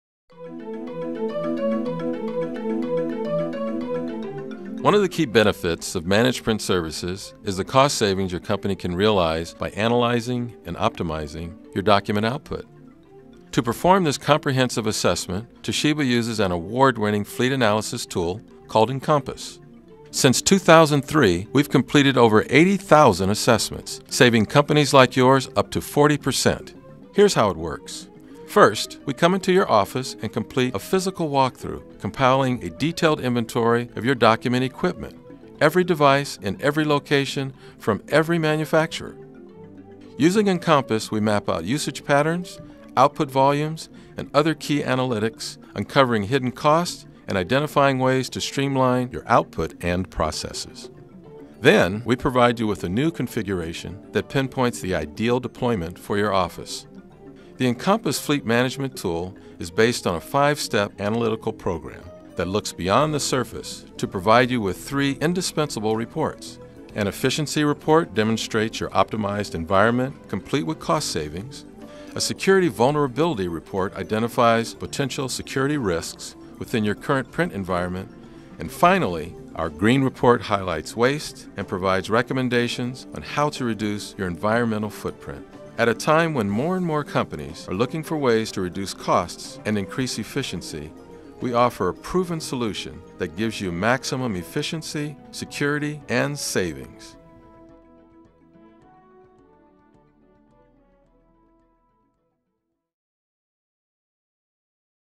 Encompass
An overview of Toshiba's Encompass software for Managed Print Services.